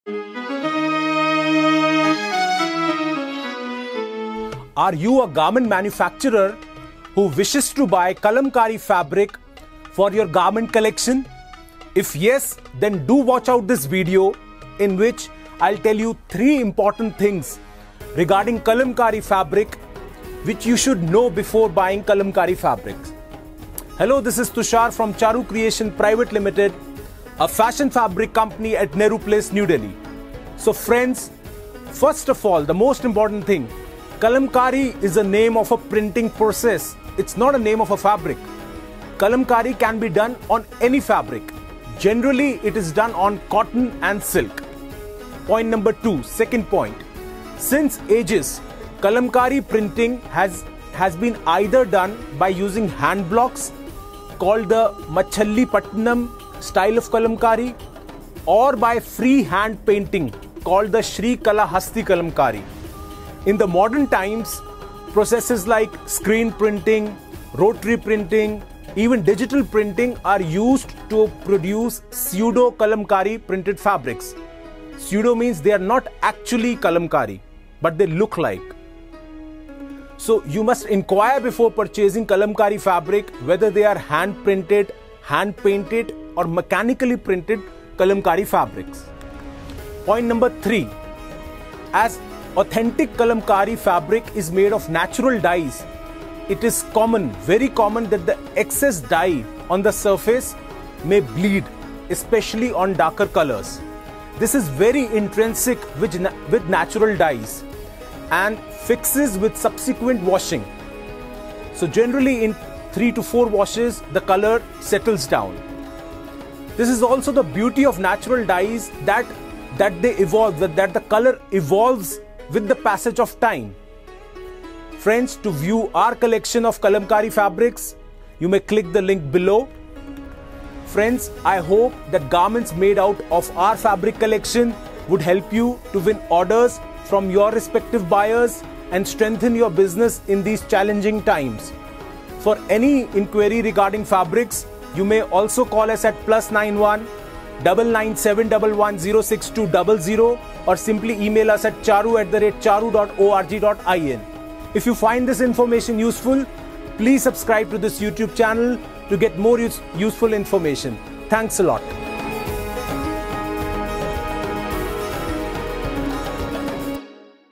3 Important Things About Kalamkari Fabric
Showroom Adress- GF-2/90, Mansarovar Building, Nehru Place, New Delhi-110019

Are you a Garment manufacturer who wishes to buy Kalamkari fabric for your Garment Collection? If yes, then do watch out this video in which I will tell you 3 important things regarding Kalamkari fabric which you should know before buying Kalamkari fabrics. Hello, this is Tushar from Charu Creation Pvt Ltd, a fashion fabric company at Nehru Place, New Delhi.

So friends, first of all, Kalamkari is a name of a Printing Process. Kalamkari can be done on any fabric. Generally, it is done on cotton and Silk

Second Point: Since ages, kalamkari printing has been either done by using hand blocks called the  Machilipatnam style of Kalamkari or by free hand-painting called the Srikalahasti Kalamkari. In the modern times, processes like Screen Printing, Rotary Printing, Digital Printing are used to produce pseudo Kalamkari Printed Fabrics. So, do enquire before purchasing Kalamkari fabric whether they are hand printed, hand painted or mechanically printed Kalamkari fabrics.

Third Point: As authentic Kalamkari fabric is made of natural dyes, it is common that the excess dye on the surface may bleed especially on darker colors. This is intrinsic with natural dyes and fixes with subsequent washing. This is also the beauty of natural dyes that they evolve with the passage of time.
To view our collection of Kalamkari Fabrics, you may click the

Friends, I hope that garments made out of our fabric collection would help you to win orders from your respective buyers and strengthen your business in these challenging times. Thanks a lot